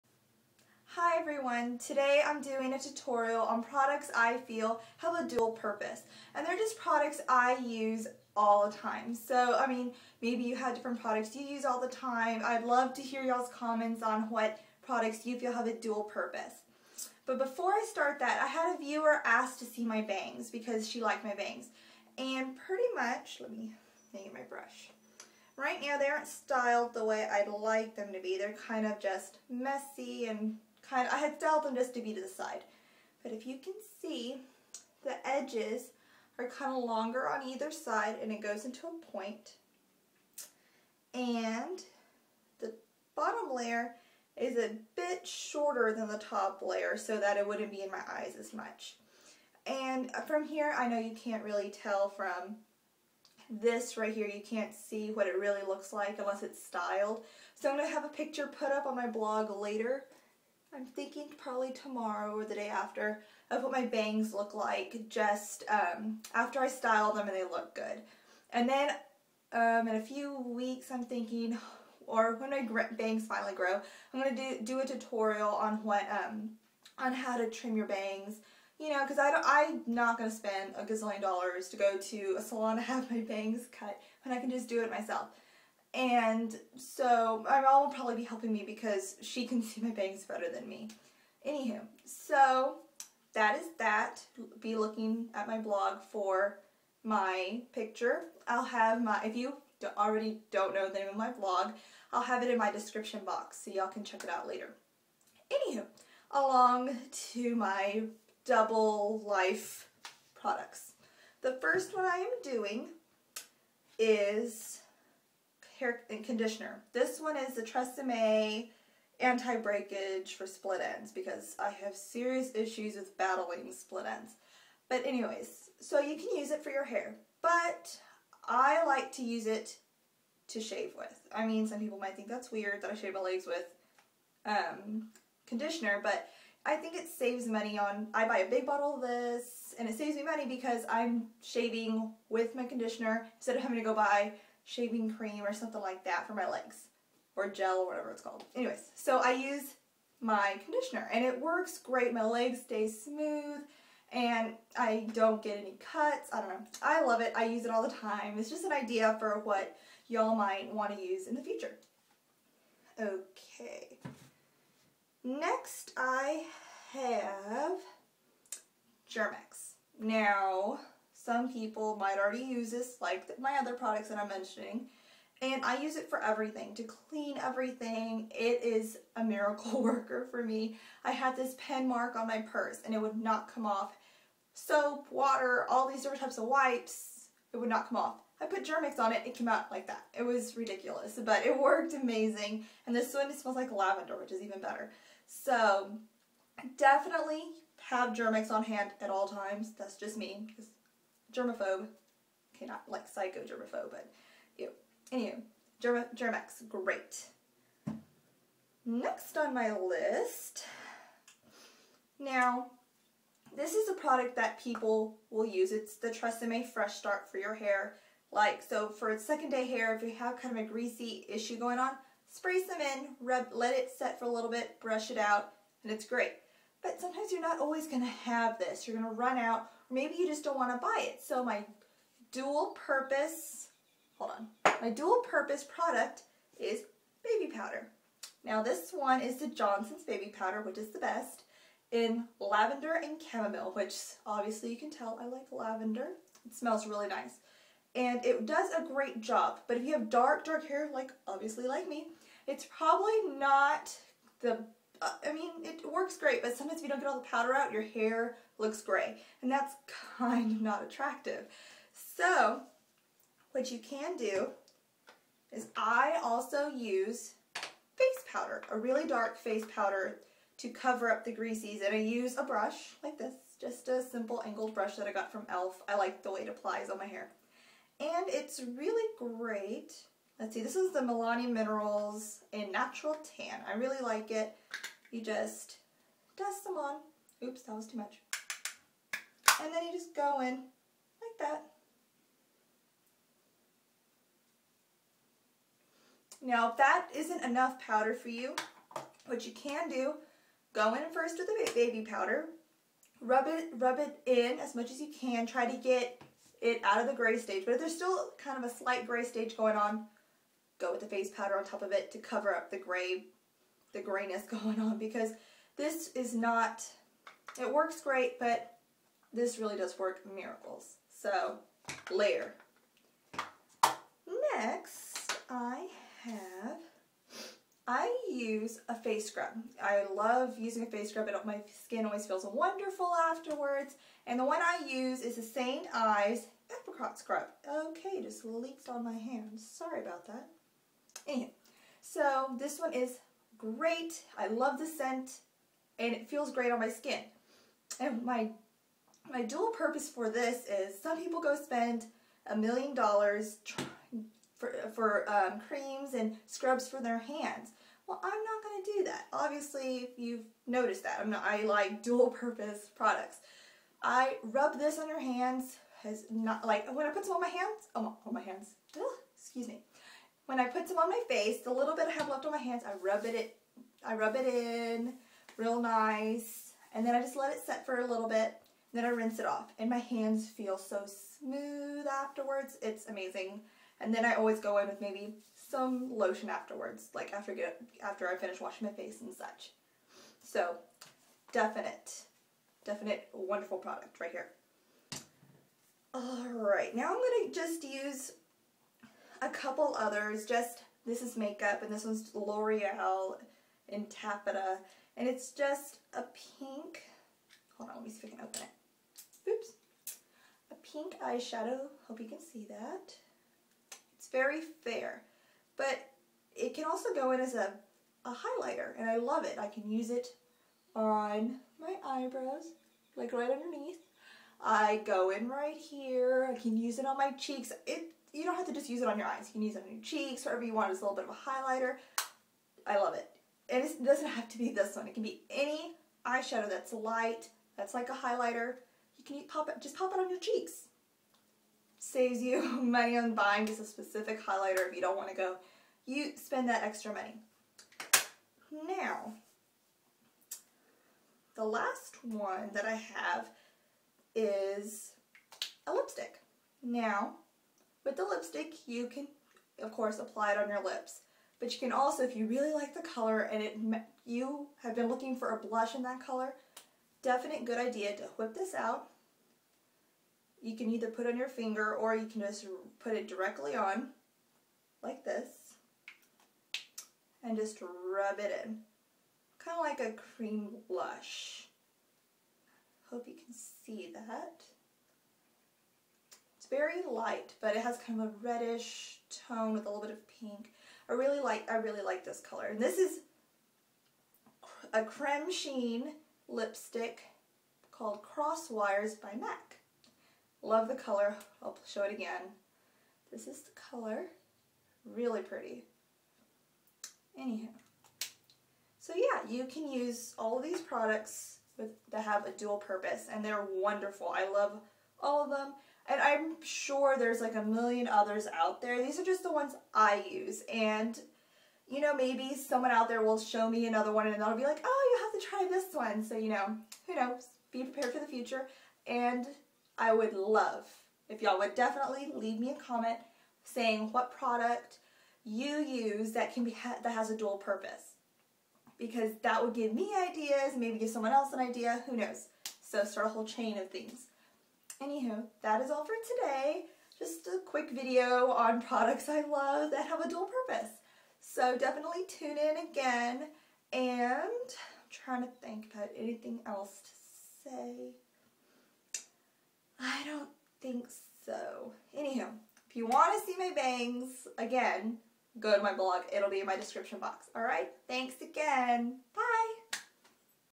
Dual Purpose Products
I talk about products I feel you can use for other uses besides the ones they r intended for.